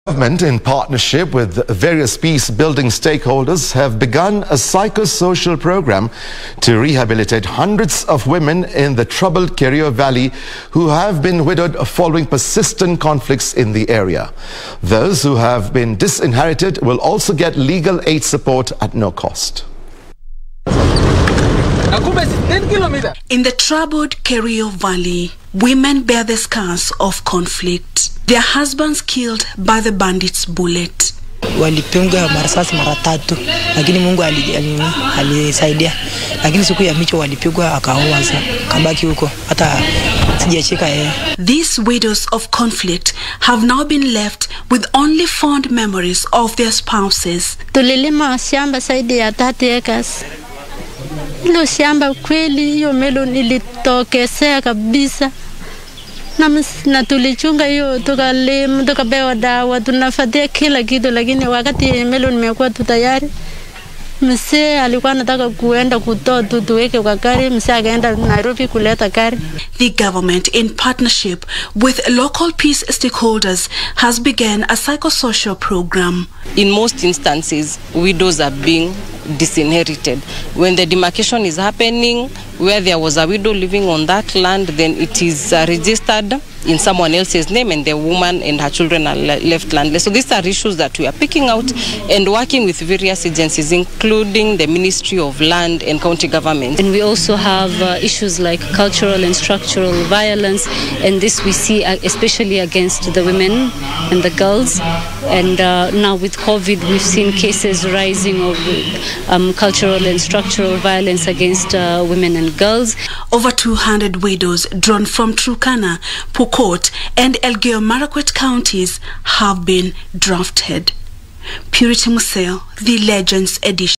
The govt begins a psycho social program to rehabilitate widows in Kerio Valley
The government in partnership with various peace building stakeholders  have began a psychosocial program to rehabilitate hundreds of women in the  troubled Kerio Valley who have been widowed following persistent conflicts in the area. Those  who have been disinherited  will also get legal aid support at no cost.